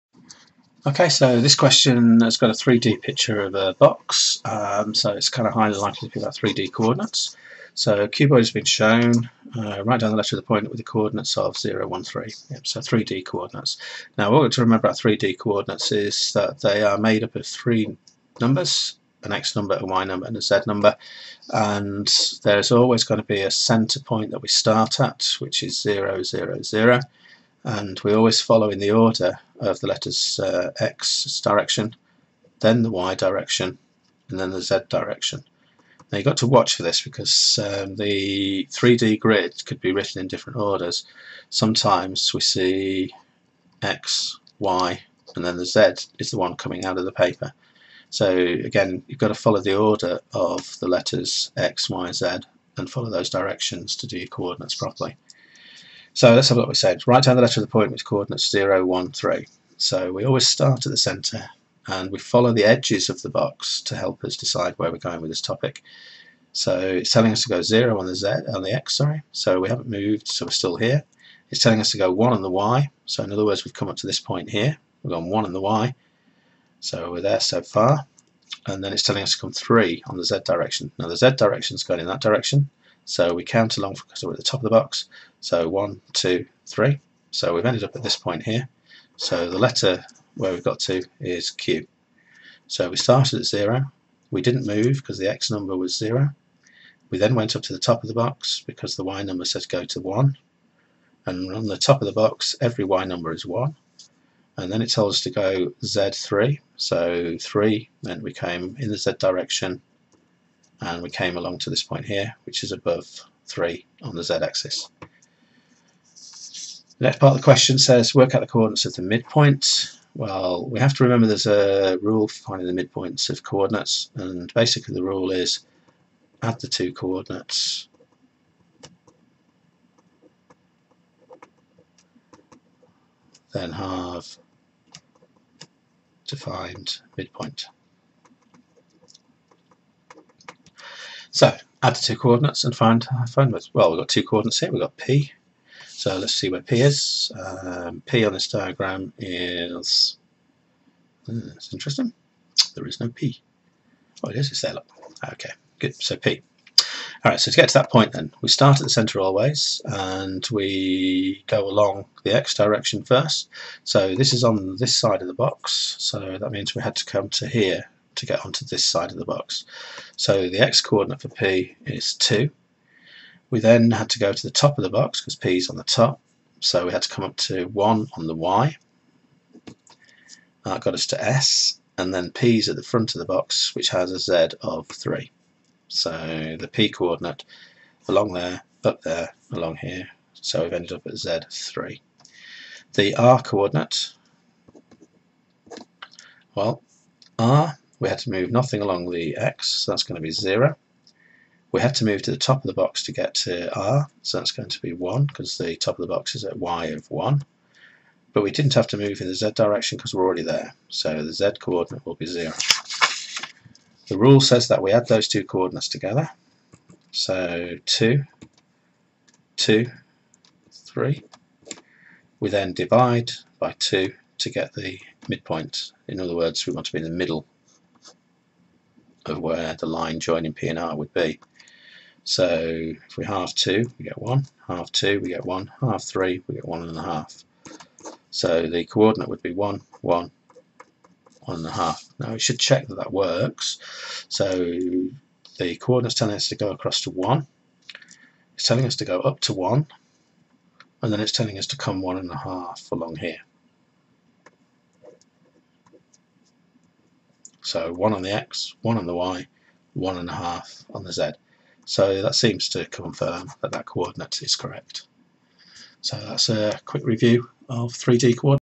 3D Coordinates and Midpoints
Finding 3D Coordinates and the Midpoint of a Line Segment in 3D